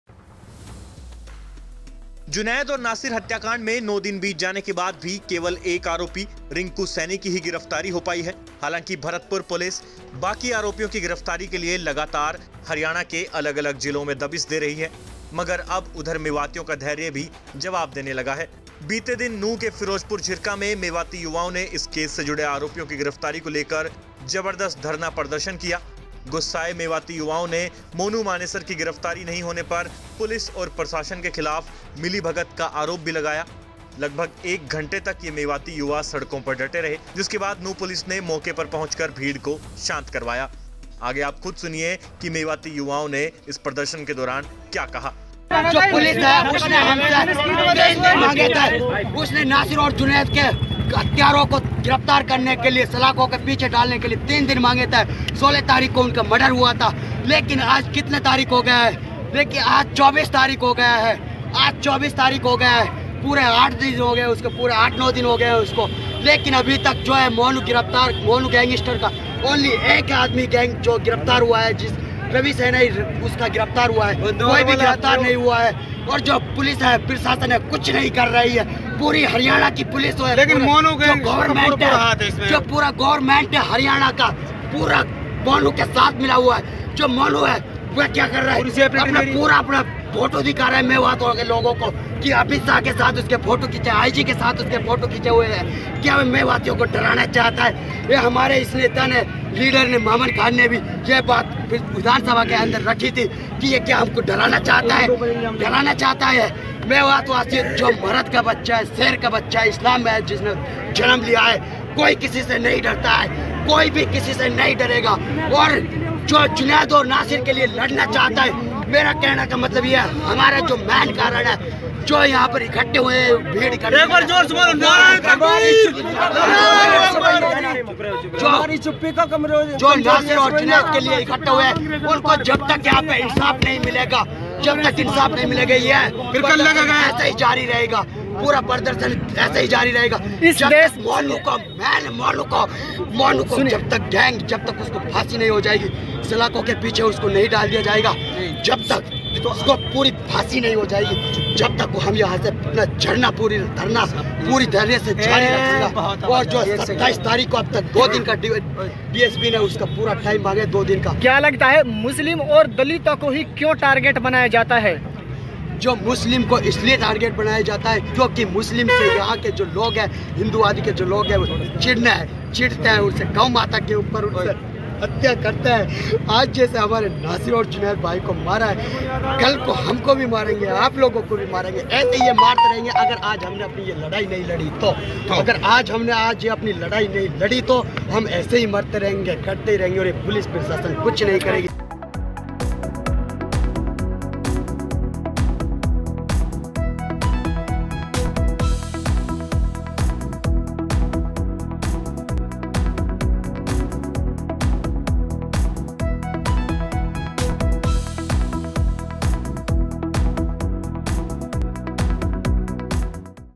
Monu Manesar की गिरफ्तारी को लेकर सड़कों पर उतरे Mewati युवा, सरकार को कड़ी चेतवानी। Haryana Tak
Monu Manesar की गिरफ्तारी को लेकर सड़कों पर उतरे Mewati युवा, सरकार को कड़ी चेतवानी।